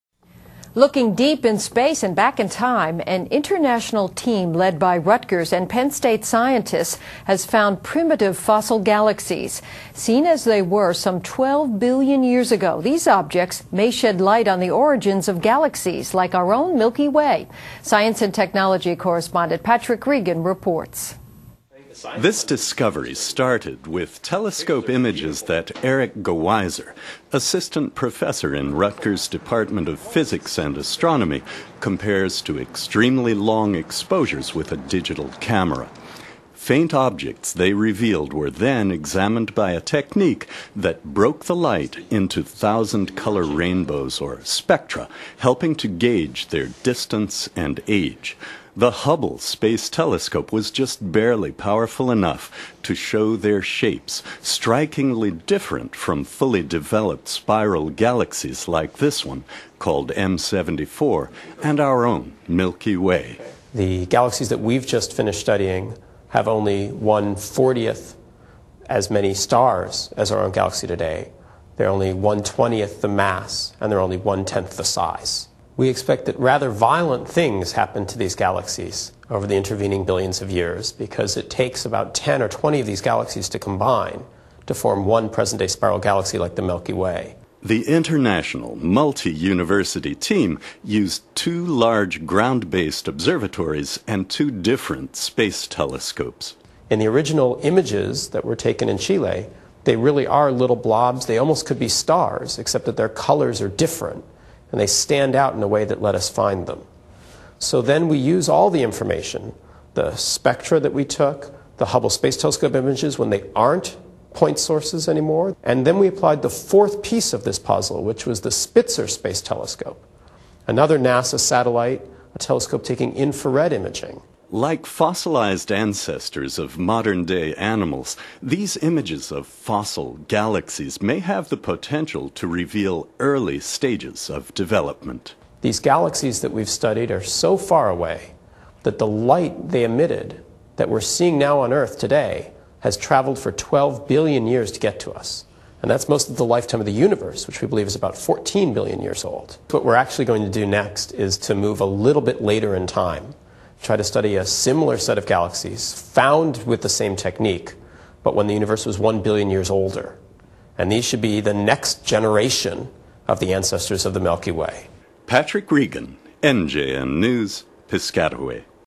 NJN News Science Report - Fossil Galaxies
Rutgers and Penn State scientists lead a international team looking deep in space and back in time, finding primitive fossil galaxies that might help explain the origin of our own galaxy.